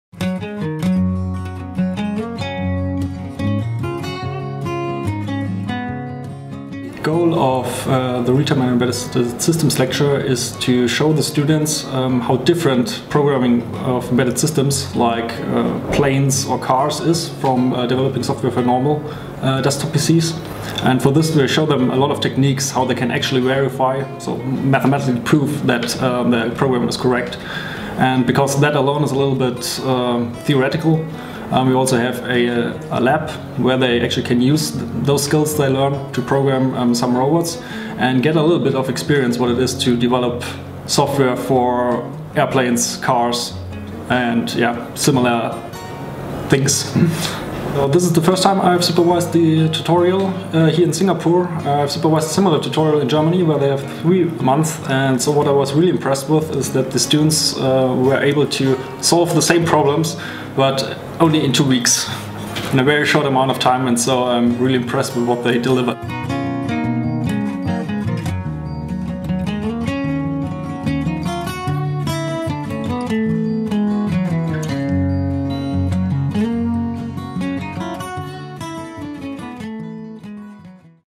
Embedded Systems (BSc Electrical Engineering & Information Technology)
We got behind-the-scenes to find out what it takes to complete a module in our Bachelor (BSc) Degree in Electrical Engineering and Information Technology. The Embedded Systems module aims to help our engineers-to-be to understand systems within automobiles and airplanes. Check out the hands-on work they do by racing pre-programmed robots against each other!